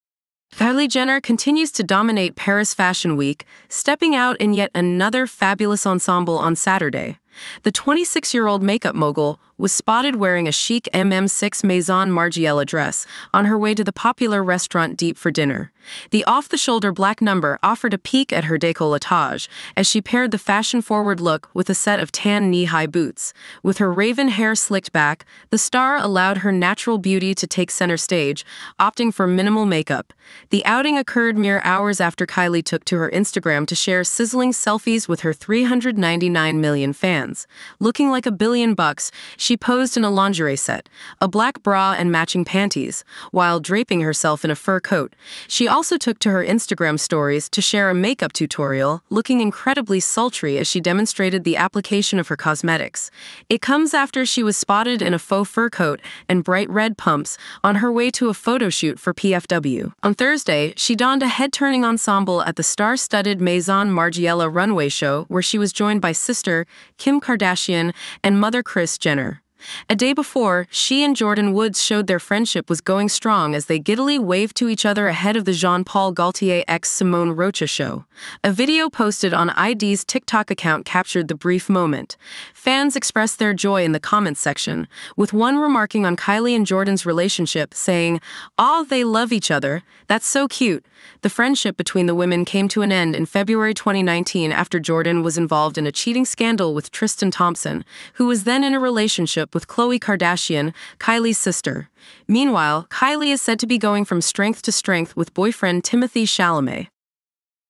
Kylie Jenner stuns in tiny strapless dress and knee-high boots for Paris Fashion Week
Kylie Jenner stuns in tiny strapless dress and knee-high boots for Paris Fashion Week... after sharing sizzling snap in black bra and matching underwear

Kylie Jenner continues to dominate Paris Fashion Week, stepping out in yet another fabulous ensemble on Saturday.

The 26-year-old makeup mogul was spotted wearing a chic MM6 Maison Margiela dress on her way to the popular restaurant Diep for dinner.

The off-the-shoulder black number offered a peek at her décolletage as she paired the fashion-forward look with a set of tan knee-high boots.

With her raven hair slicked back, the star allowed her natural beauty to take center stage, opting for minimal makeup.

The outing occurred mere hours after Kylie took to her Instagram to share sizzling selfies with her 399 million fans.

Looking like a billion bucks, she posed in a lingerie set - a black bra and matching panties - while draping herself in a fur coat.

She also took to her Instagram Stories to share a makeup tutorial, looking incredibly sultry as she demonstrated the application of her cosmetics.

It comes after she was spotted in a faux fur coat and bright red pumps on her way to a photo shoot for PFW.

On Thursday, she donned a head-turning ensemble at the star-studded Maison Margiela runway show where she was joined by sister, Kim Kardashian, and mother Kris Jenner.

A day before, she and Jordyn Woods showed their friendship was going strong as they giddily waved to each other ahead of the Jean Paul Gaultier x Simone Rocha show.

A video posted on i-D's TikTok account captured the brief moment.

Fans expressed their joy in the comments section, with one remarking on Kylie and Jordyn's relationship, saying, 'Awww, they love each other; that's so cute.'

The friendship between the women came to an end in February 2019 after Jordyn was involved in a cheating scandal with Tristan Thompson, who was then in a relationship with Khloe Kardashian, Kylie's sister.

Meanwhile, Kylie is said to be going from strength to strength with boyfriend Timothee Chalamet.

The loved-up couple put their chemistry on display at the recent Golden Globe awards.

'Timothee mingled for a little bit but was otherwise right next to Kylie the whole time,' a source told Us Weekly.

'They were having a very intimate conversation and shared a few kisses. It was obvious they have amazing chemistry.'

The duo initially sparked speculation about their romance back in April, following their rendezvous a few months prior at Paris Fashion Week.

Timothee has even met Kylie's two kids. She shares Stormi, five, and Aire, 23 months, with her ex-boyfriend Travis Scott, 32.

'Timothee has met them in group situations where there were other family members or friends around,' a second source for the outlet revealed

'It hasn’t been anything where it’s just Kylie, Timothee and the kids in an intimate setting. And she introduced him as one of Mommy’s friends. Kylie feels like it’s [still] too soon to let them know anything about their relationship.'Kylie Jenner stuns in tiny strapless dress and knee-high boots for Paris Fashion Week